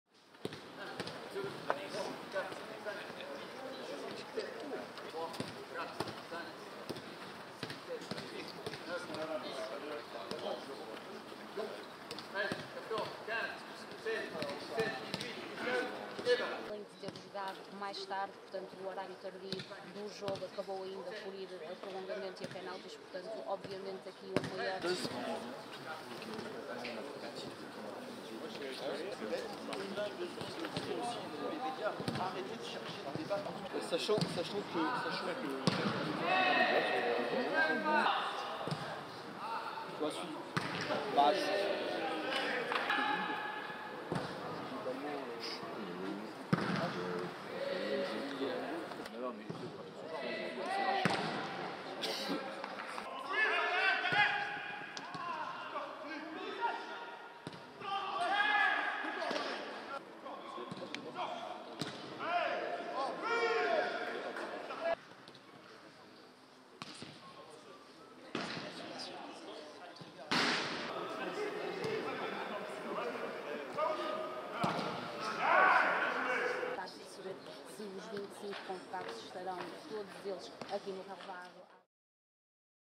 كأس أوروبا 2024: فرنسا تستعد لمواجهة البرتغال في ربع النهائي | AFP
أجرى لاعبو المنتخب الفرنسي الذين لم يشاركوا في التشكيلة الأساسية أمام بلجيكا حصة تدريبية استعداداً لمواجهة البرتغال مساء الجمعة في ربع نهائي كأس أوروبا 2024 التي تحتضنها ألمانيا.